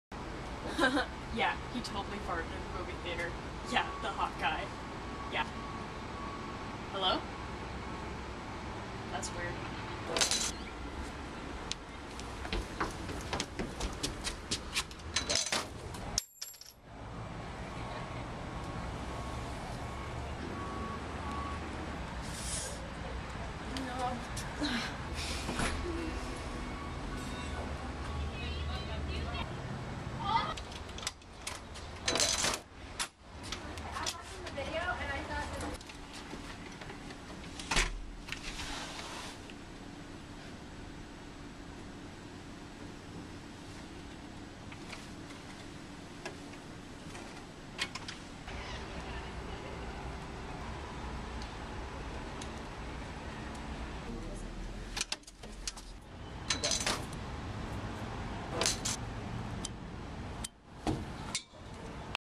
door scene
Because I thought I had the best door scene but it was not chosen, so I made it anyway.